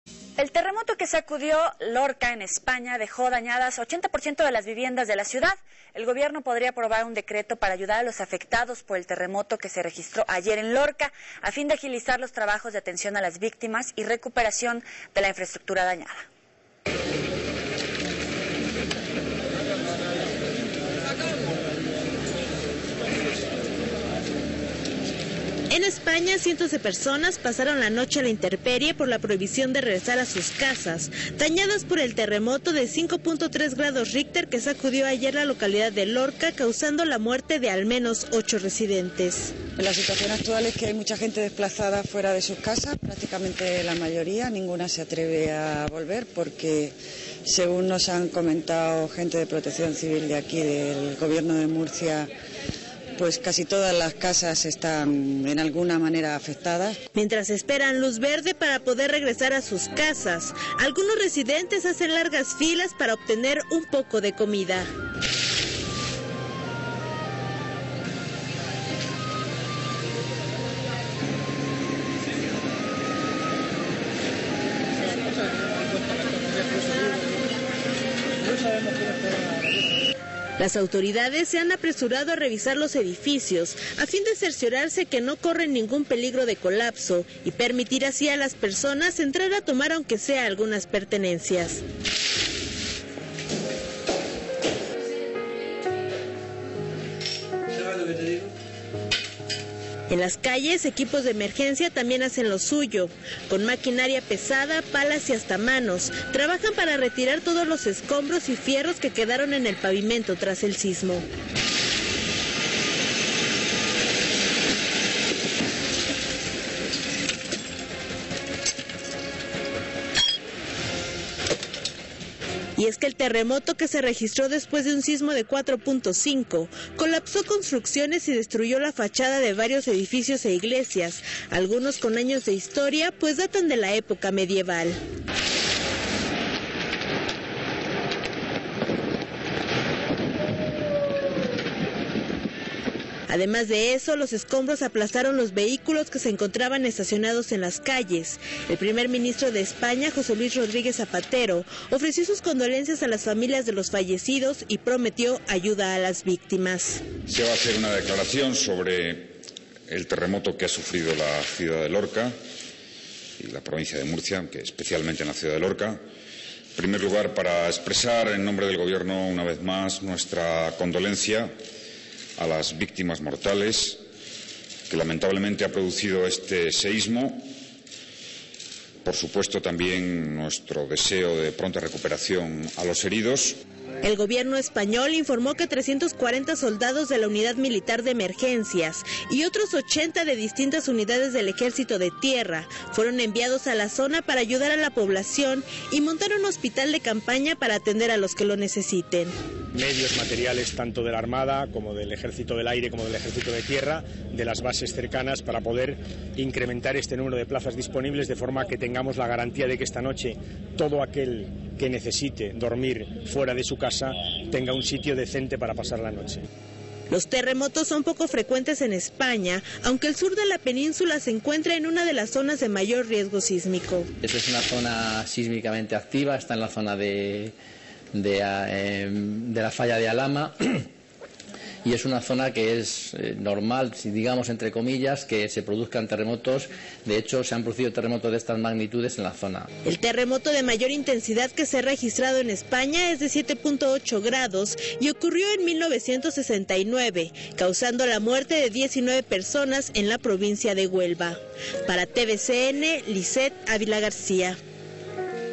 Terremoto en España
El terremoto que sacudió la localidad de Lorca, en España dejó dañadas el 80 por ciento de las viviendas de la ciudad...

El gobierno podría aprobar un decreto para ayudar a los afectados por el sismo, a fin de agilizar los trabajos de atención a las víctimas y recuperación de la infraestructura dañada...